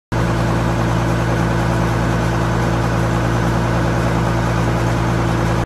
1997 CASE IH 8950 For Sale
MFWD, 250 HP, PTO, Power Shift Transmission, 3 Pt hitch w Quick hitch, 4 Remotes, Rear Duals Front tires 420/90R30, Rear 20.8/R42 tires.  Front and inside rear wheel weights.  Extended front weight bracket.  All lights operational, AC blows cool, starts right up, ready to get to work!